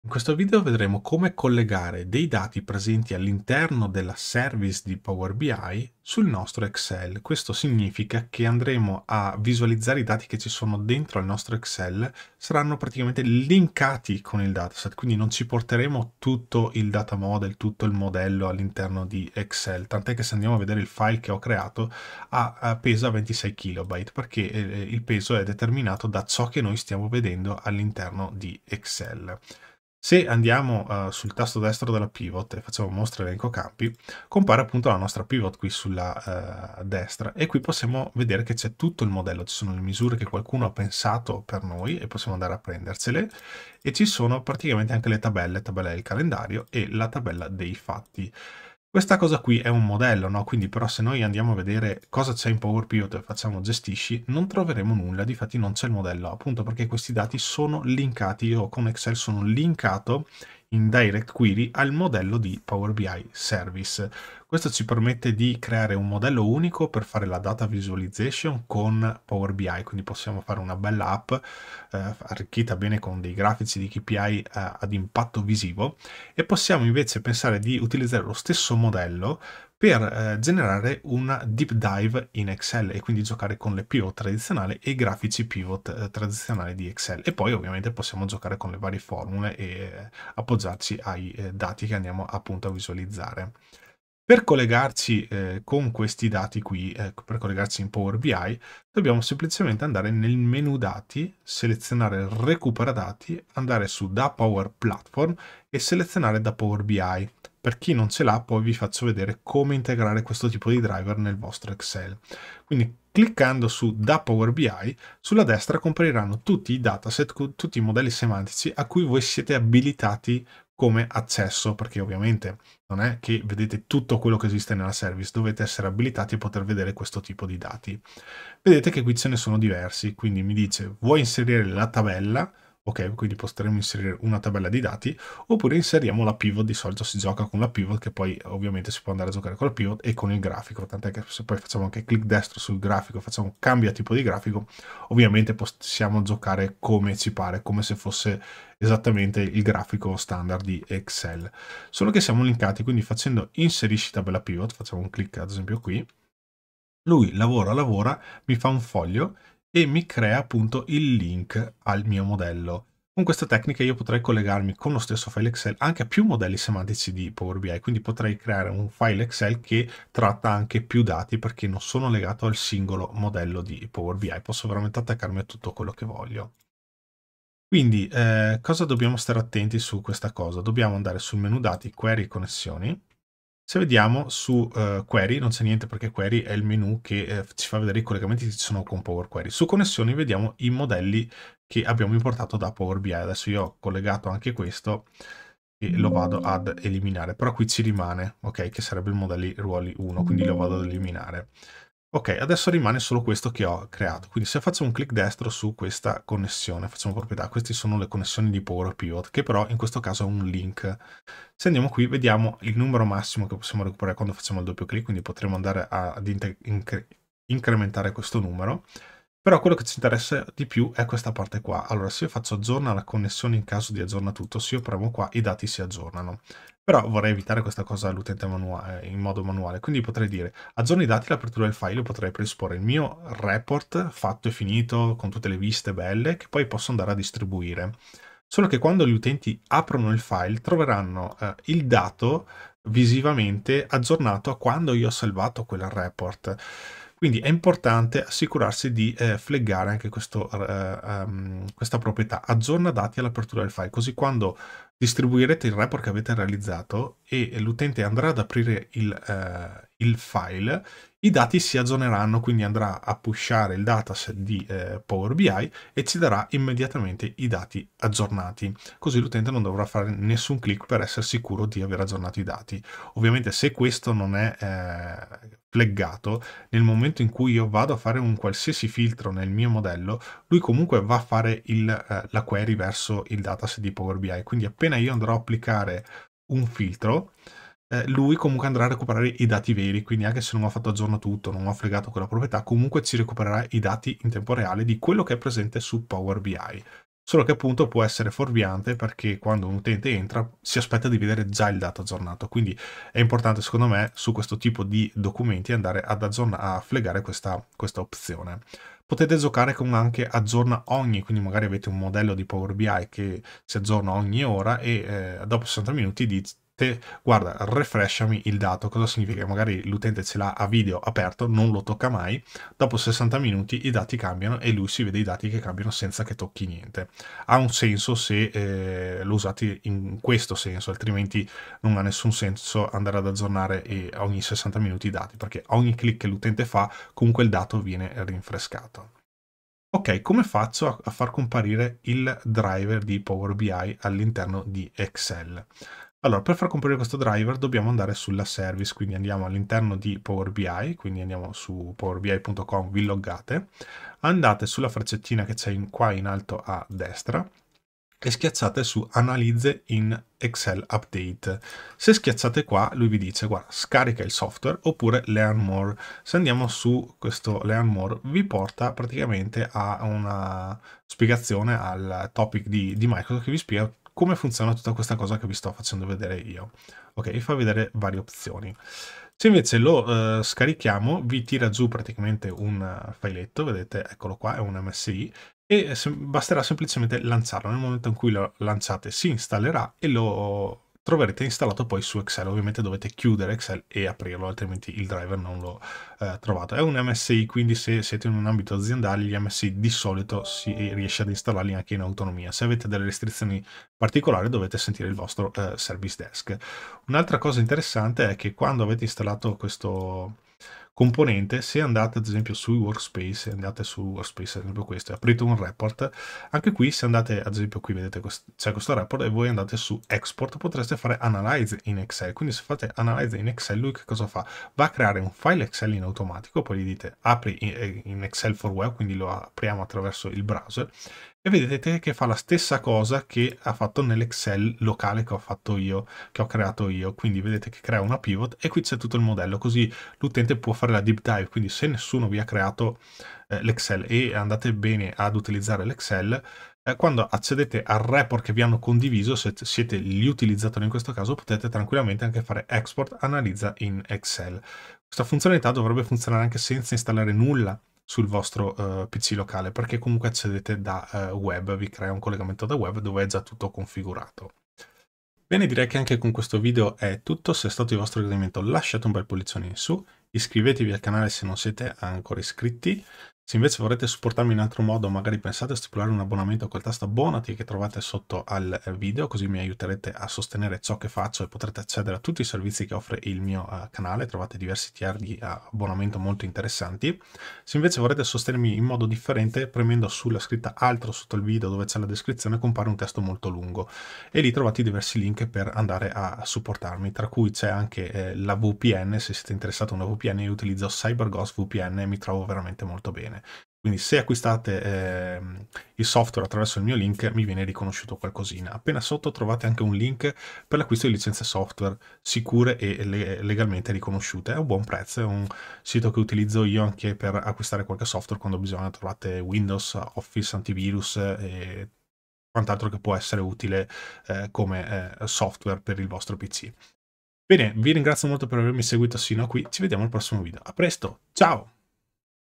Linkare PBI ad Excel
In questo video scopriremo come eseguere un linked connection con i dati presenti in un dataset di powerbi, in Excel. Quindi nessun dato sarà importato in Excel, tutti i dati che saranno esposti o interrogati verranno recuperati in tempo reale dal modello semantico di PowerBI.